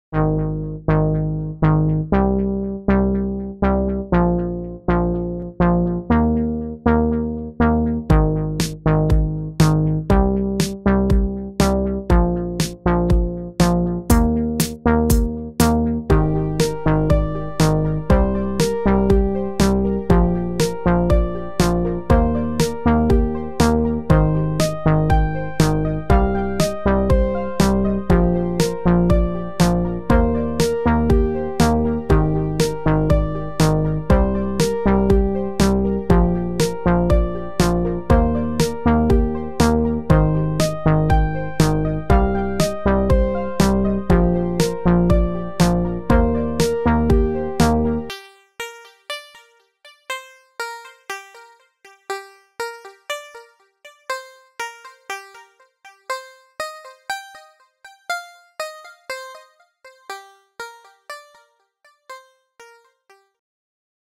Parker V Brock
Procreate art tutorials in under a minute! Spider-Man versus Venom. Lame art and lamer beats by Rubin Pingk.